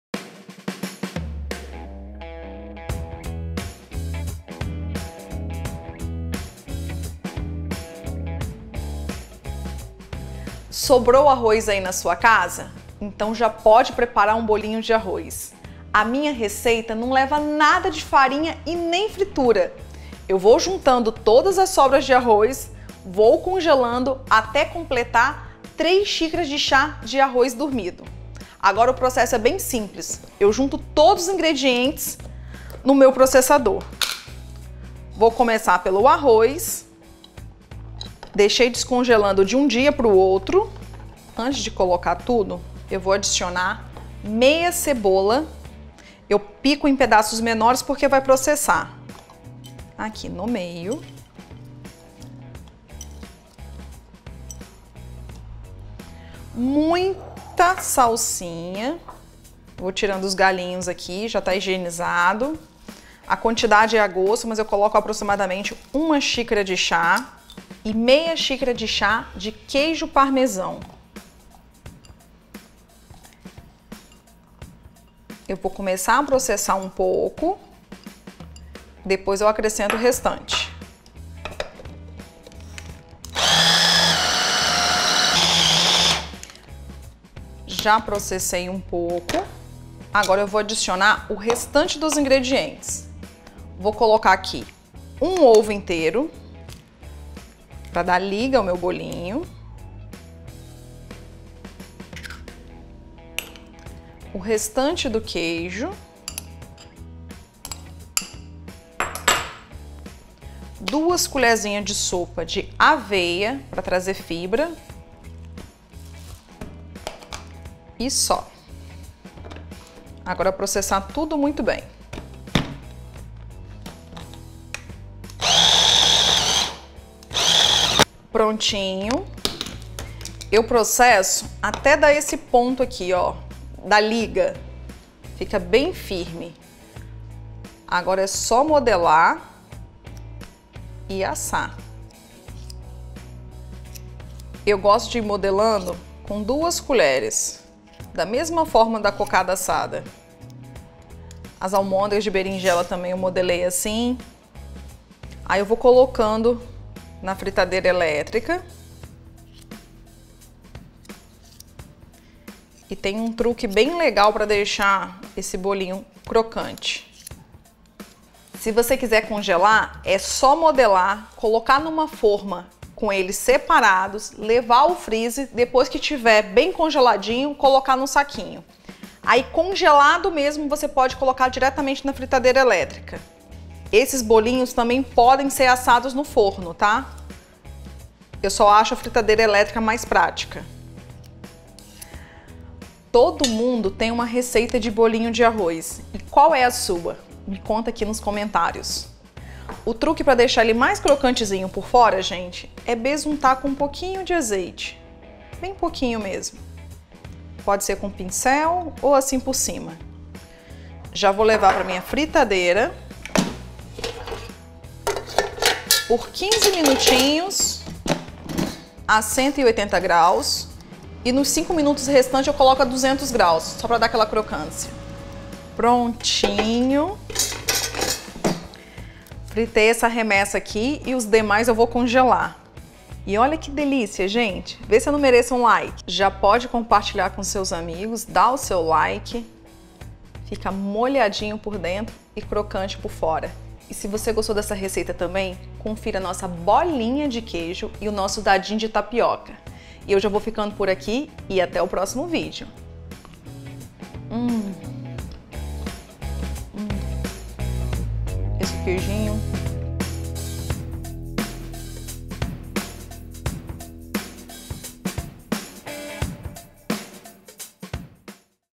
BOLINHO DE ARROZ SEM FARINHA E SEM OLÉO | PRÁTICO E RÁPIDO | CULINÁRIA DESCOMPLICADA
Sempre sobra um restinho de arroz do almoço ou jantar. Pode ir juntando para prepararmos a minha receita, bem prática e rápida, de bolinho de arroz. Não utilizo nenhuma farinha e nem frito em óleo. Fica macio por dentro e crocante por fora. Confira também as dicas de congelamento.

🔴 PRODUTOS EM PROMOÇÃO PARA A SUA COZINHA

Ingredientes do BOLINHO DE ARROZ:
3 xícaras (chá) de sobras de arroz cozido
1/2 cebola picada
Salsinha ou cebolinha a gosto (utilizei a medida de 1 xicara de chá)
1/2 xícara (chá) de queijo parmesão, pode substituir por mussarela ou queijo minas (pode aumentar a quantidade se preferir)
1 ovo
2 colheres (sopa) de aveia em flocos (opcional)